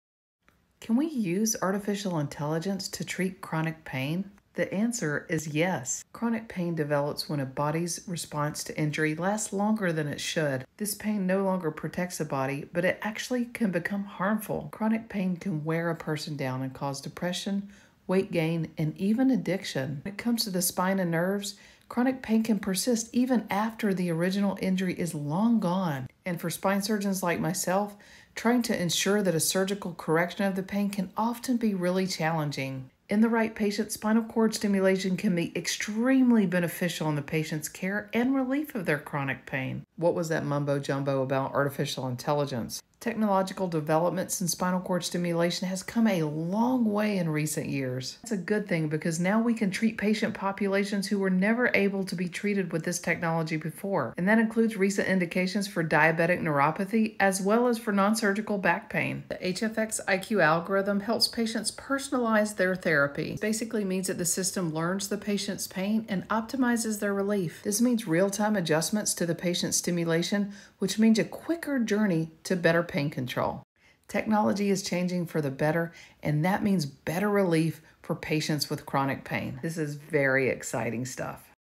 Dr. Grunch explains how artificial intelligence can be used to treat chronic pain
HFX iQ is a game-changer for people with chronic pain. Nevro’s artificial intelligence is transforming the way we approach chronic pain management. It’s a powerful tool that can help us personalize treatments, optimize outcomes, and empower patients to take control of their health.

HFX iQ uses sophisticated algorithms and machine learning to adapt to your body’s changing needs and optimize pain relief. It has been shown to provide significant pain relief, reduce the need for medication, and improve overall function.

Let’s use the power of technology to make chronic pain a thing of the past.